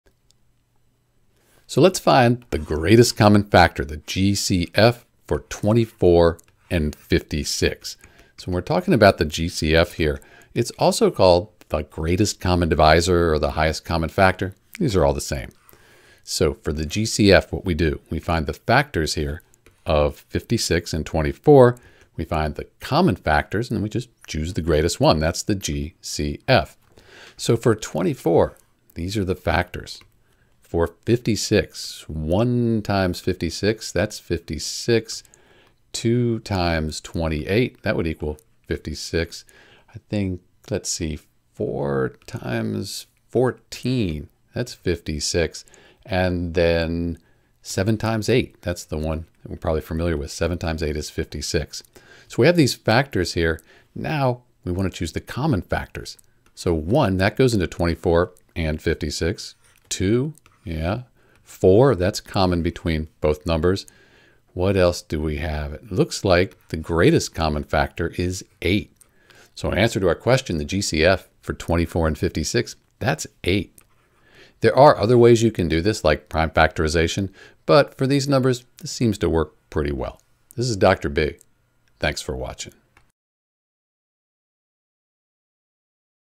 How to Find the Greatest Common Factor for 24 and 56
• The factors of 24 are the numbers that can evenly divide 24. In this case, they are 1, 2, 3, 4, 6, 8, 12, and 24.

• The factors of 56 are the numbers that can evenly divide 56. In this case, they are 1, 2, 4, 7, 8, 14, 28, and 56.

Common Factors: 1, 2, 4, 8